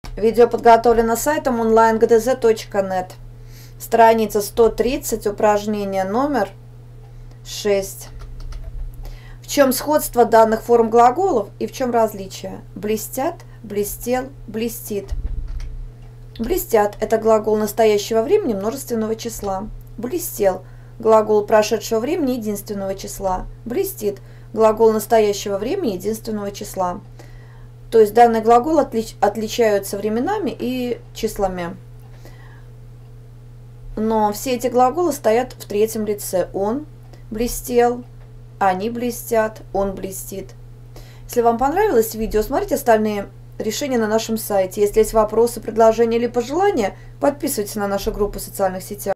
Задания проверь себя (6) для главы Глагол - Русский язык 3 класс (Канакина, Горецкий) Часть 2
Если вы хотите верно сделать упражнение №6 на странице 130, то нужно уметь определять, в чем сходство и различие в данных форм глаголов. Если вы хотите выполнить это задание быстро и без ошибок, мы рекомендуем к просмотру наше видео!